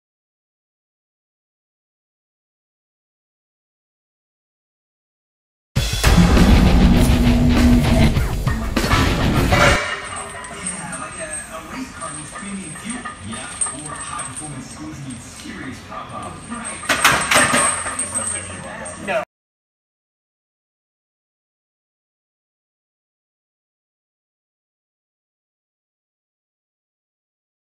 Elitefts Sponsored Powerlifter Brian Schwab 2 Board Bench in the Metal Catapult
Elitefts Sponsored Powerlifter Brian Schwab benching 275+80 lbs of chains off of a 2 Board in the Metal Catapult.